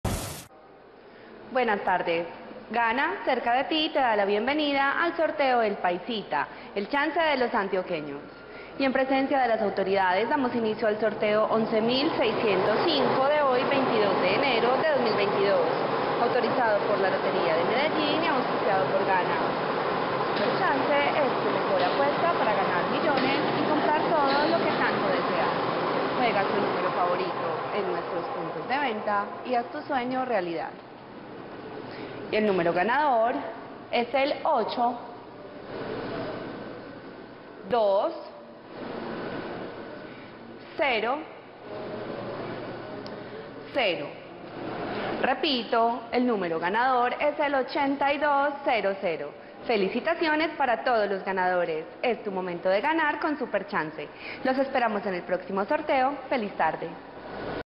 EL PAISITA 2 Resultados del Chance del Sábado 22 de enero de 2022 ✅🥇🔥💰
Hola buenas noches, te presentamos los resultados de las loterías y chances en Colombia, vamos con el Resultados del Chance El Paisita 2 del Sábado 22 de enero de 2022.